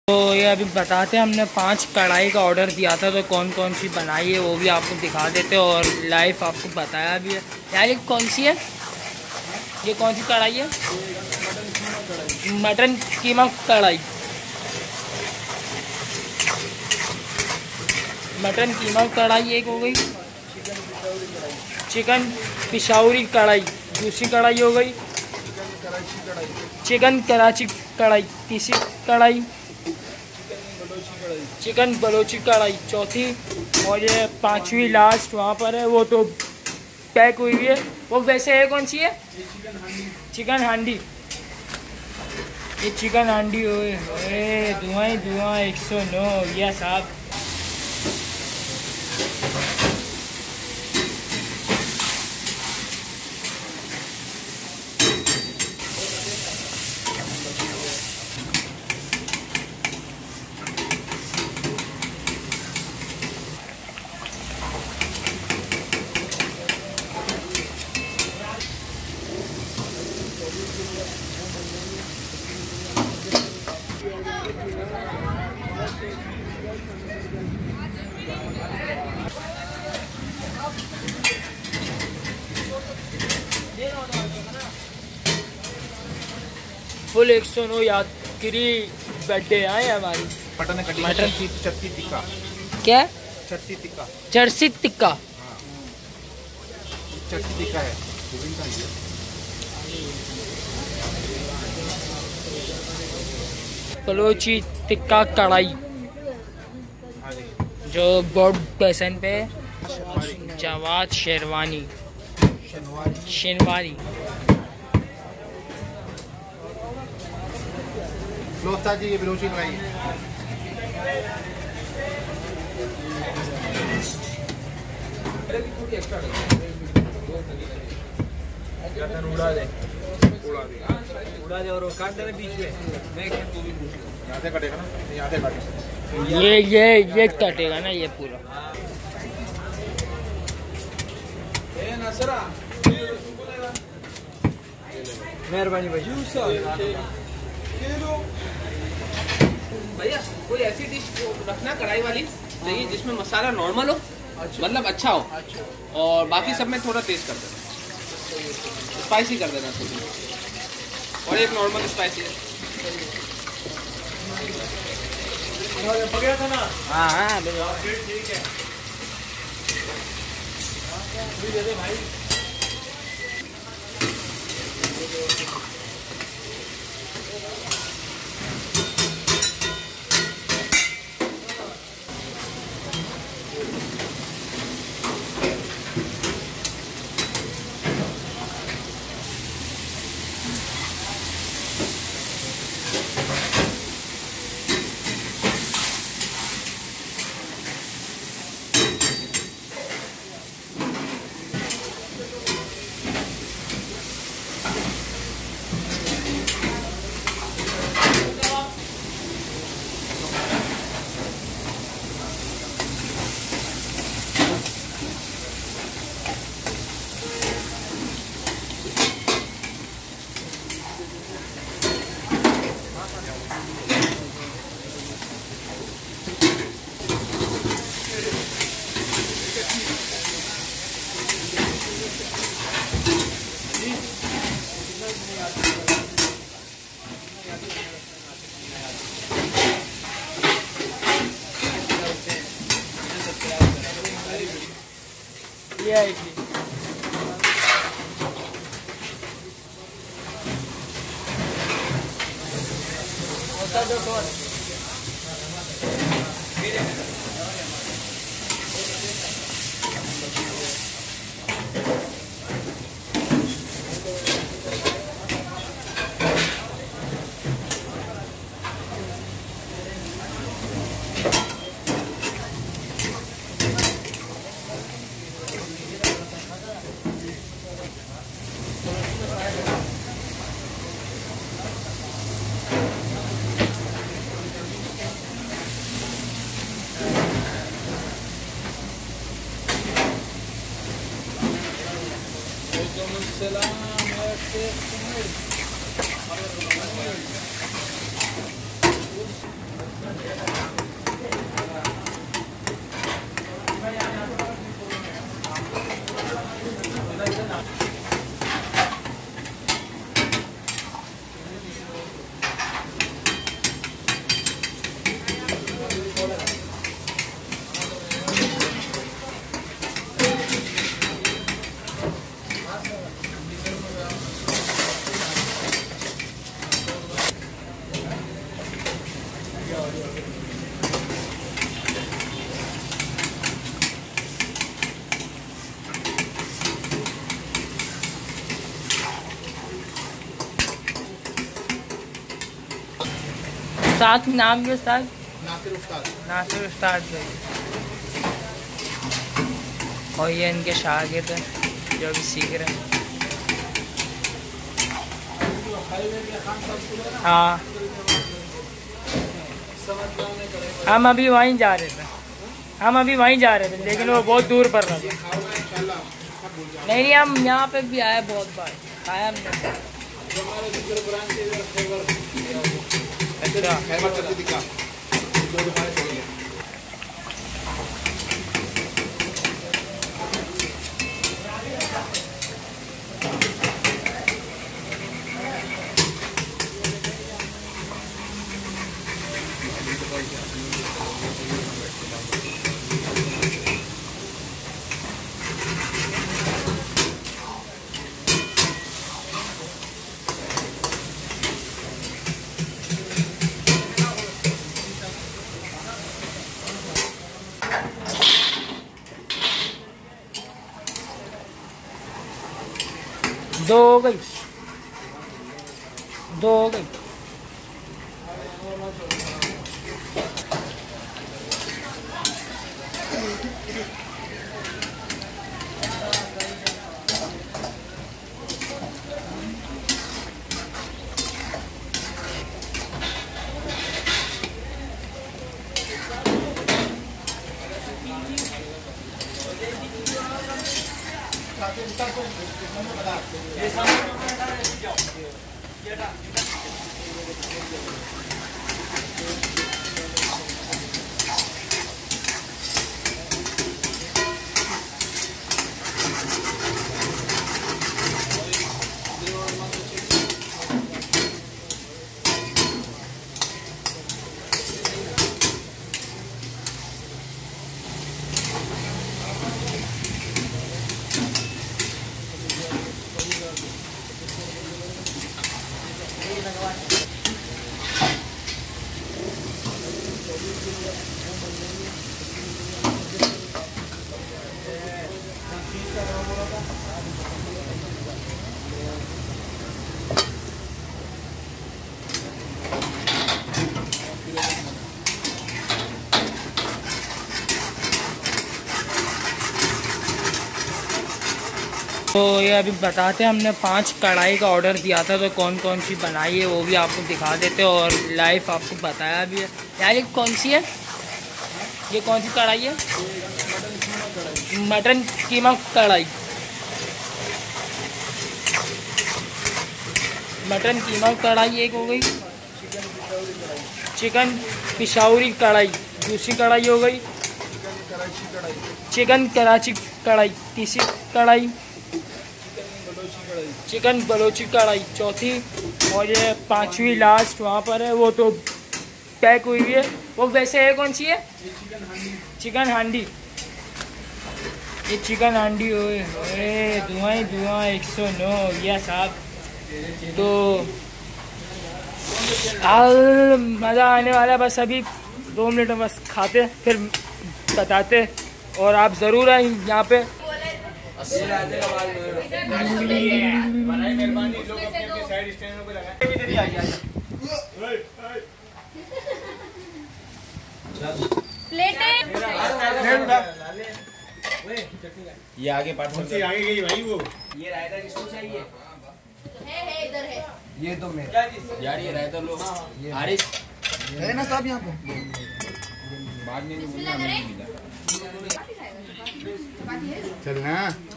Peshawari Chicken Karahi | Balochi Tikka & Charsi Tikka Karahi at Shinwari Food Street of Boat Basin
Peshawari Chicken Karahi | Mutton Karahi | Balochi Tikka Karahi & Charsi Tikka Karahi at Jawed Shinwari Food Street of Boat Basin!